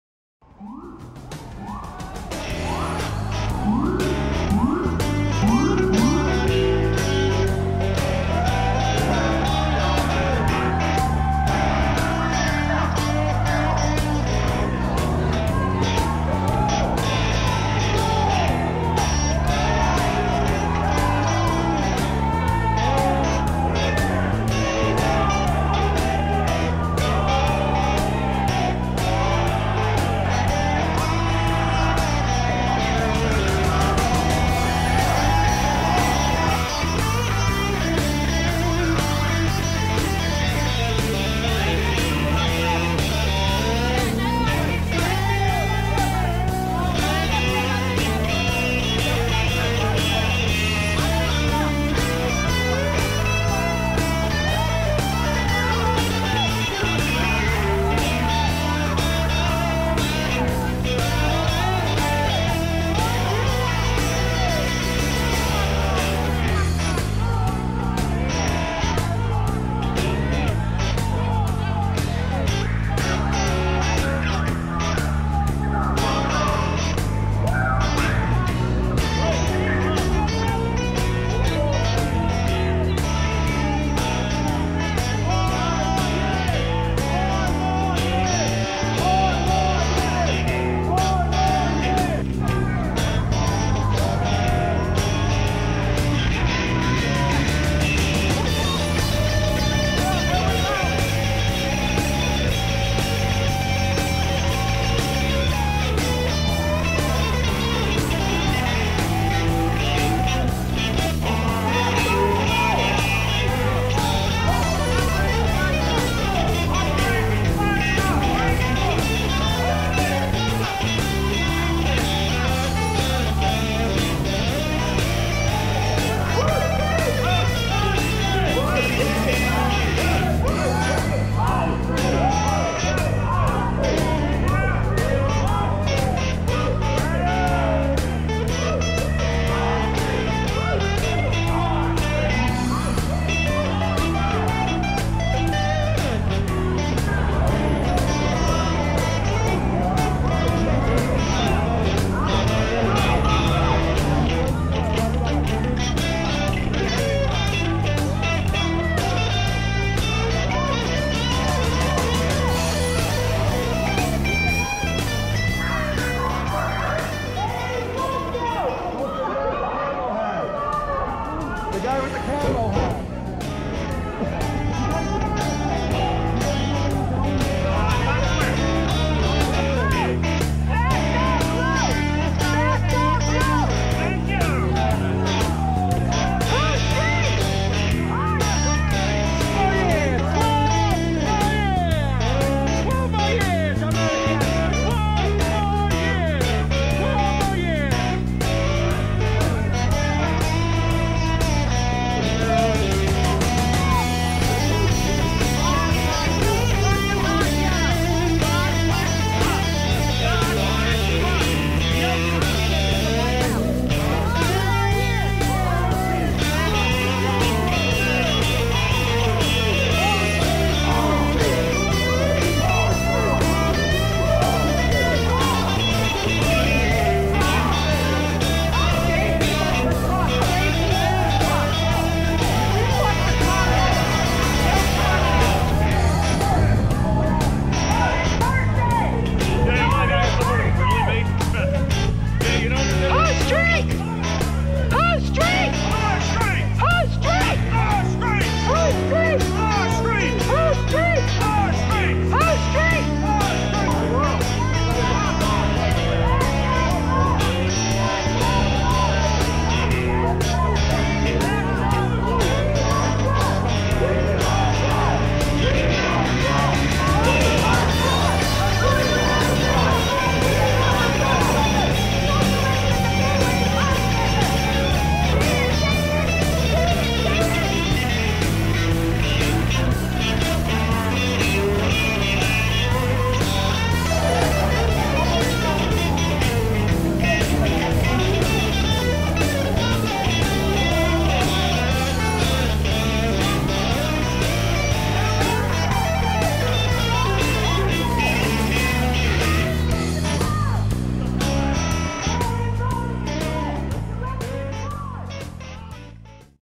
Dion Cini Operation Flag Drop October 13 2020
World's Largest Trump Flag unveiling,
Courtesy of  NTD. Enjoy! REPOST
Operation Flag Drop